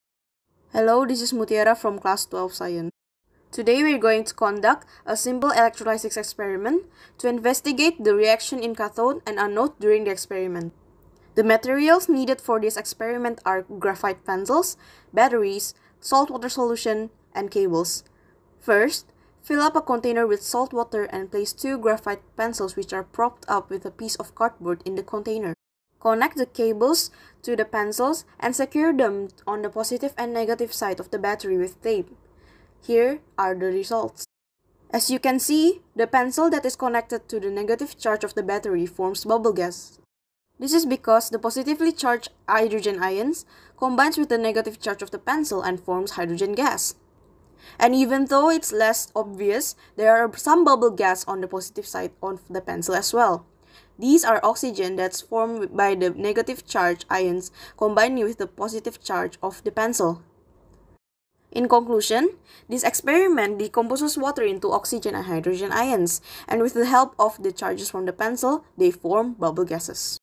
Electrolysis of Dilute Sodium Chloride by Carbon
Hello everyone, in this video Mutiara from Grade 12S, Saint Nicholas School did an electrolysis experiment from home.
the electrolyte: dilute sodium chloride
the electrode is :Carbon